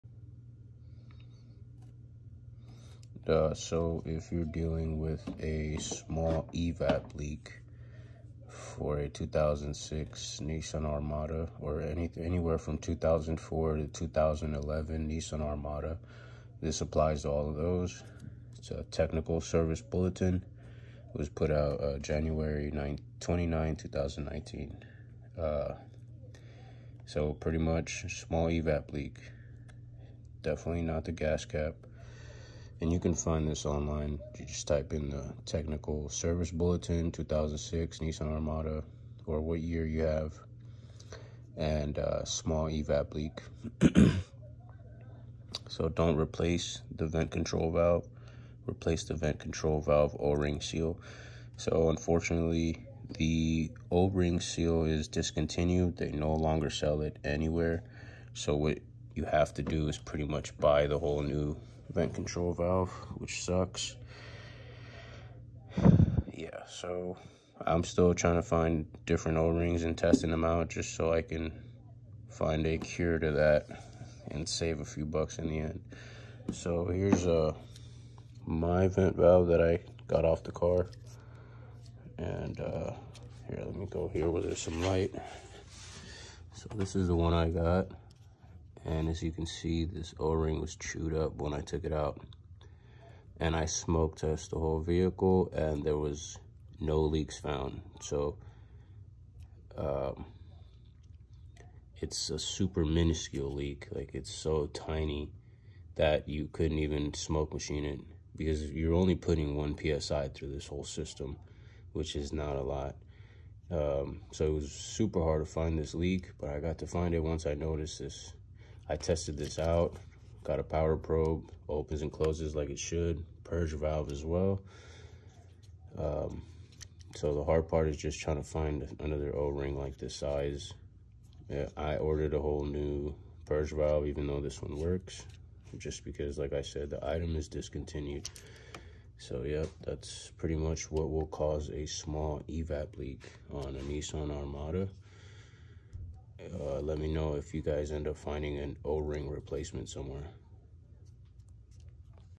Common small EVAP leak code on Nissan Armada & Technical Service bulletin
Smoke testing Nissan Armada 2006 for evap leaks due to small evap leak code, after inspecting found out it was the vent control valve O-ring